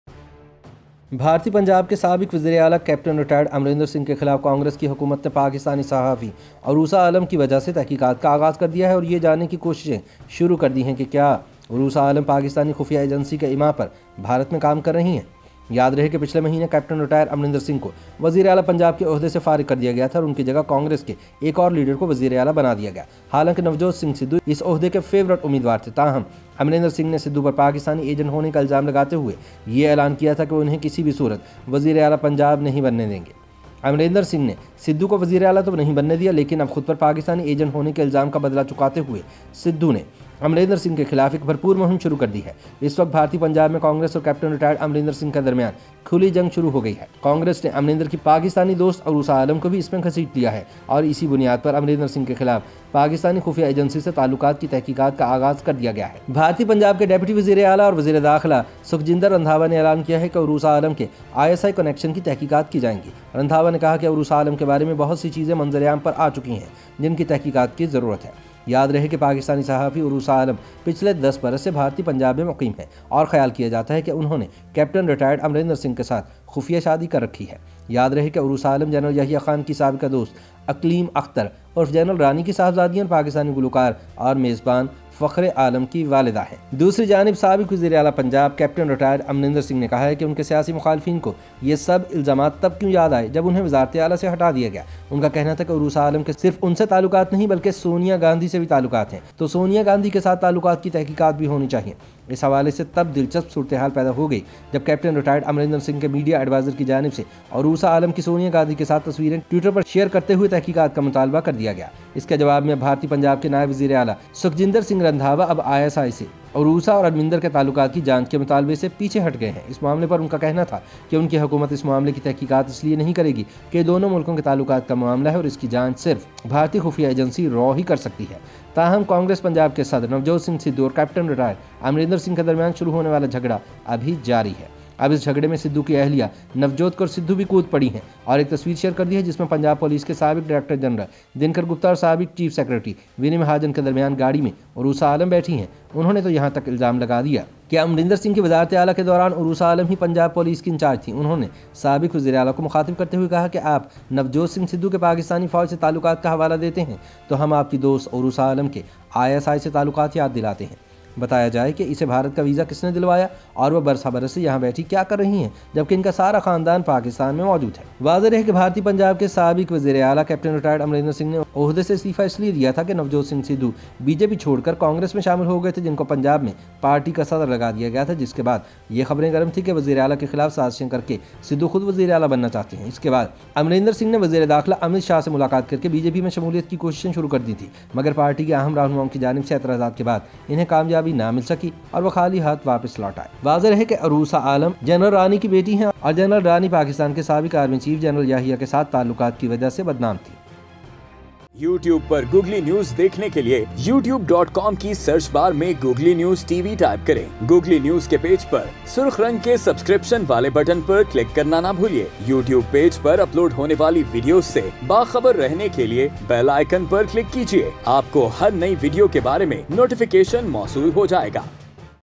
Pakistani Sahafi Aroosa Alam Ka Naam India Mein Kiyun Goonjnay Laga? | Googly News TV
Pakistani Sahafi Aroosa Alam Ka Naam India Mein Kiyun Goonjnay Laga?